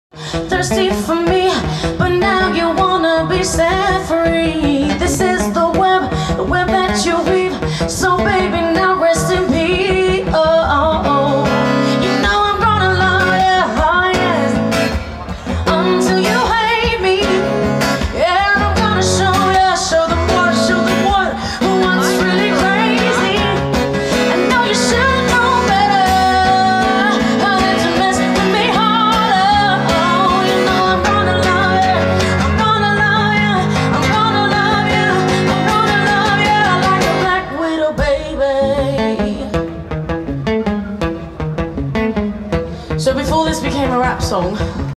Cannes lions 17
Painted the Ipg media house at the festival , Rita Ora was a guest, a good laugh and a down to earth person, cannes lions report at Dotmaster.co.uk